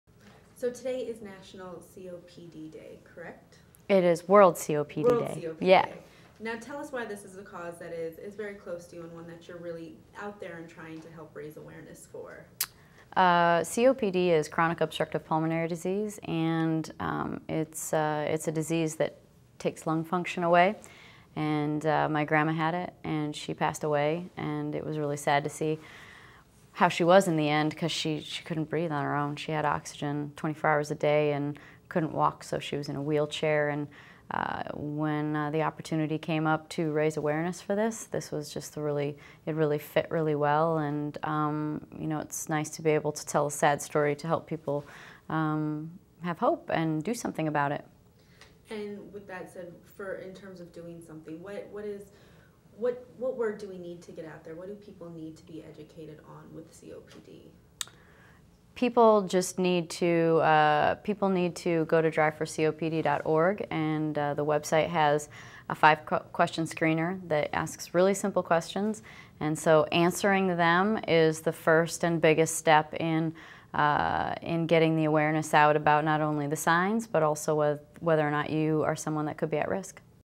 INTERVIEW: Danica Patrick on why Chronic Obstructive Pulm...
INTERVIEW: Danica Patrick on why Chronic Obstructive Pulmonary Disease is so important to her, and on what people need to know about about COPD INTERVIEW: Danica Patrick on why Chronic Obstructi at W Los Angeles - Hollywood Hotel on November 14, 2012 in Hollywood, California